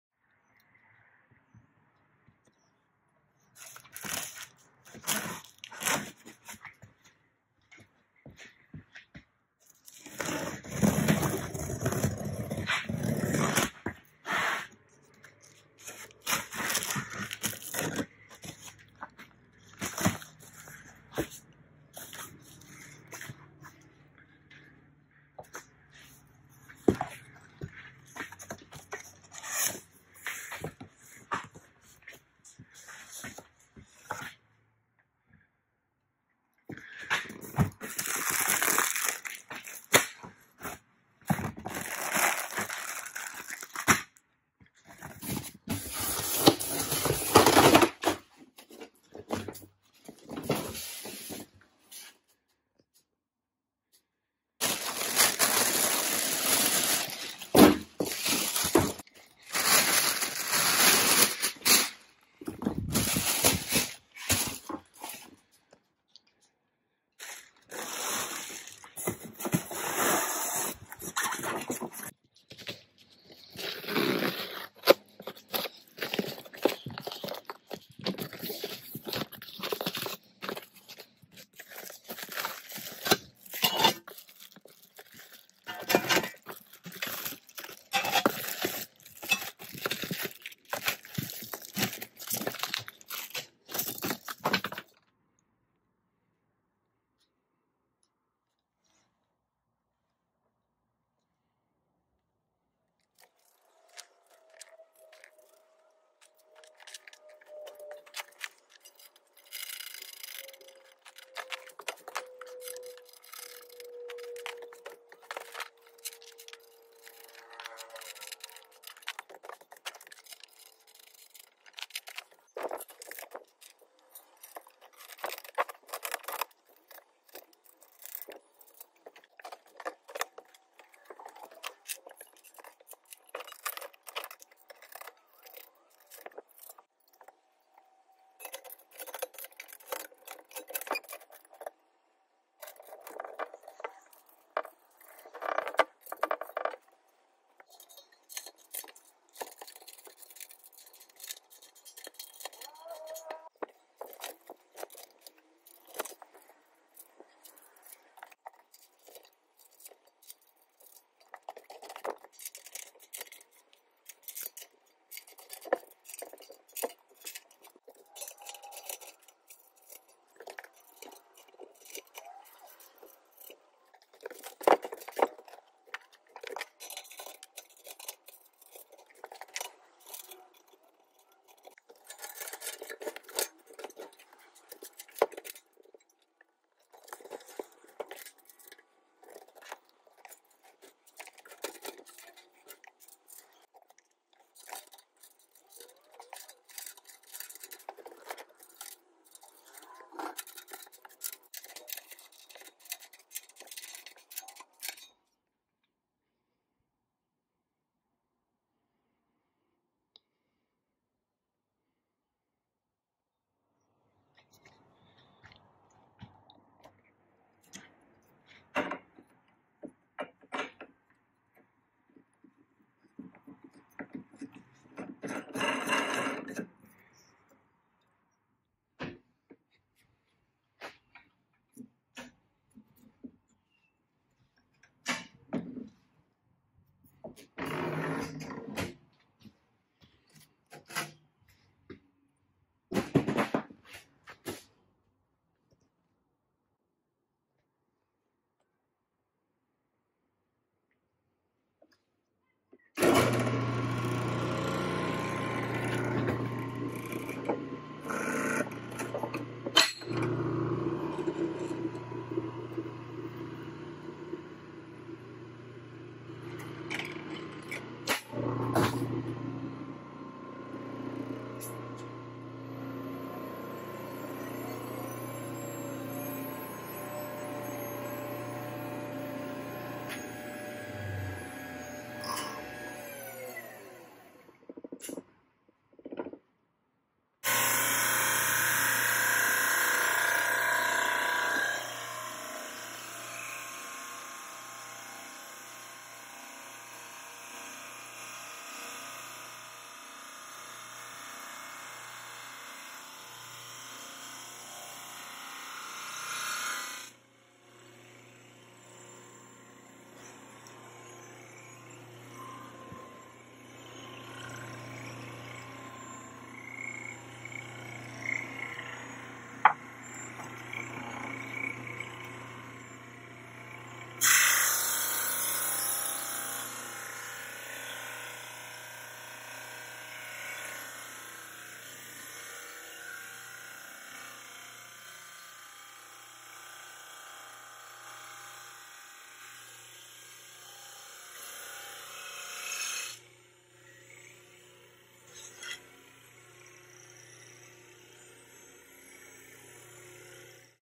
I Bought a Drum Sander! Unboxing and Setting up my Supermax 16-32
I purchased a new Supermax 16-32 drum sander from Busy Bee Tools and thought I'd make a short video of unboxing it and getting it set up.